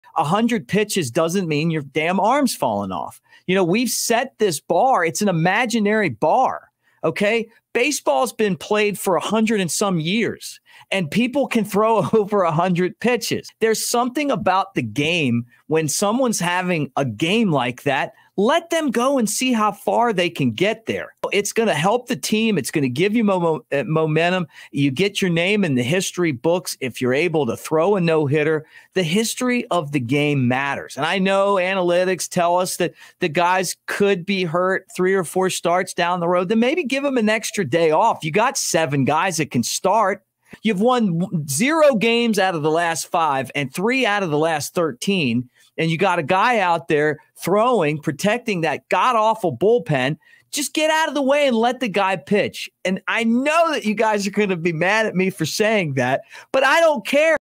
Chicago Cubs Ben Brown Pulled During No-Hitter Pitch Counts vs History
Mick Gillispie gives his take on the Cubs pulling Ben Brown on May 28th needing six outs for a No-Hitter.